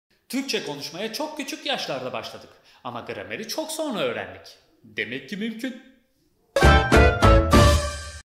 Gramer Bilmeden İngilizcemizi Geliştirmek Mümkün Mü? | Sıkça Sorulan Sorular
Ana dilimizi nasıl öğrendiğimizi hatırlayalım: Henüz hiçbir dil bilmiyorken, etrafımızda ebeveynlerimizin Türkçe konuşmalarına maruz kaldık ve zamanla kelime kelime konuşmaya başladık. Anne, baba, mama gibi... 7 yaşımıza geldiğimizde Türkçe konuşabiliyorduk. Ancak okullarımızda dil bilgisi kurallarını 3.-4. sınıflar dersleri ile öğrenmeye başladık. Yani; Türkçe konuşabiliyorduk, ancak dil bilgisi kurallarını sonra öğrendik. İngilizceyi de aynı şekilde edinebilmek elbette mümkün!